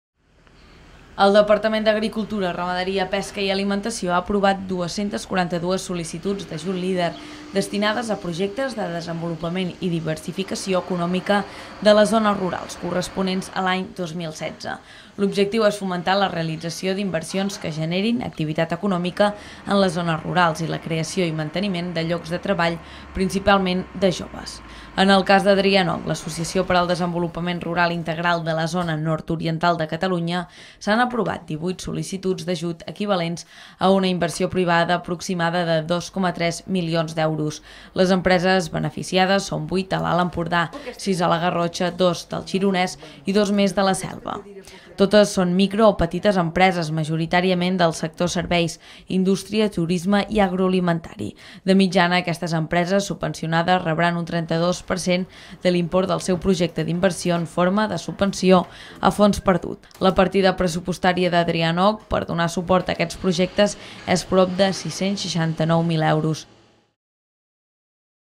Sis empreses garrotxines es beneficiaran dels ajuts Leader de desenvolupament en entorns rurals
El Departament d’Agricultura ja ha resolt la darrera convocatòria Leader d’ajuts al desenvolupament a les sones rurals. 18 empreses del nord-est de Catalunya se’n beneficiaran d’aquests ajuts, entre elles, sis empreses garrotxines.